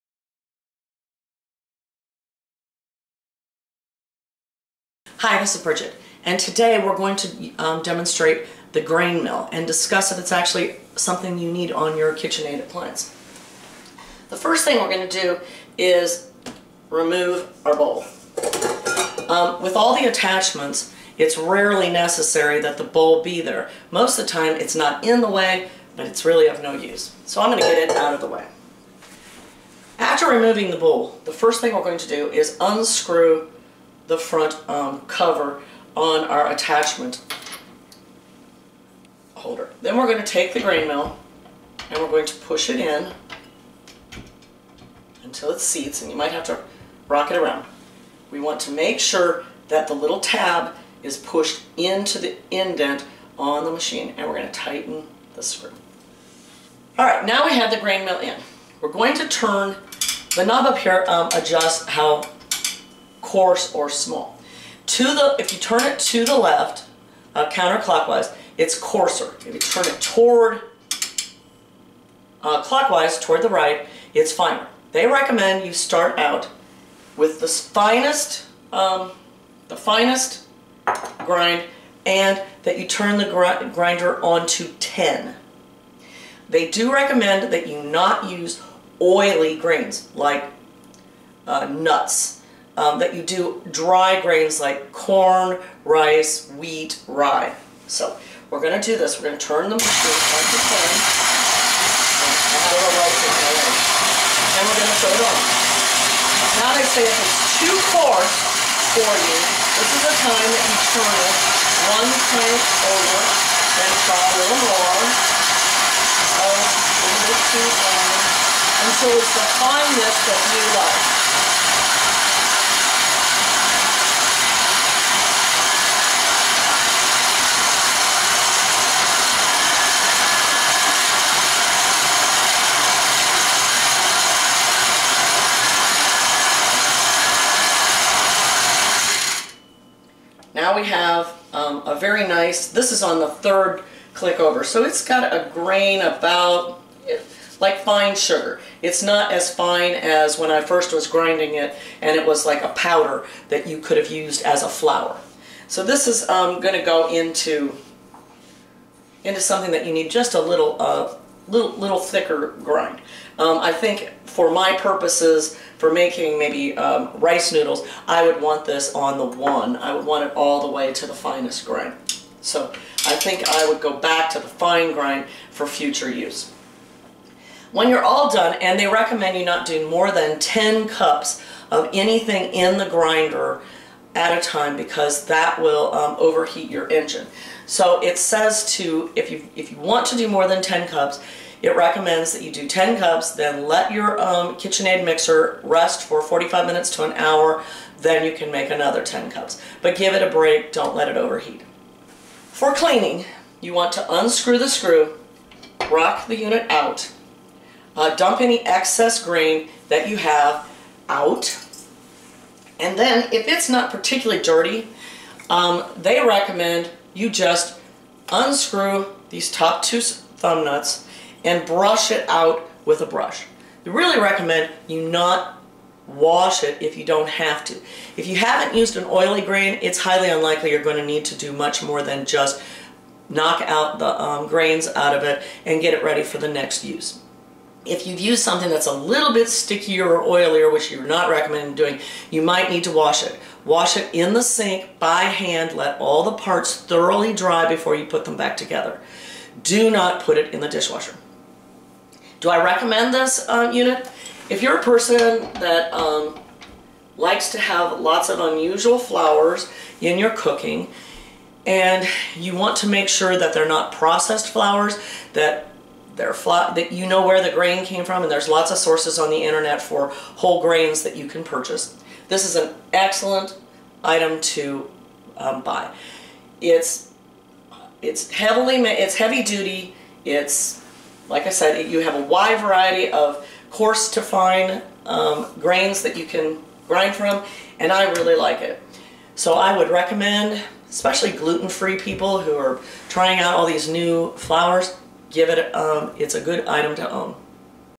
The Kitchenaid Grain Mill - what I think of it and how to use it
I love my Kitchenaid Grain Mill and think its an attachment that many people would find useful. Its fairly easy to use and clean.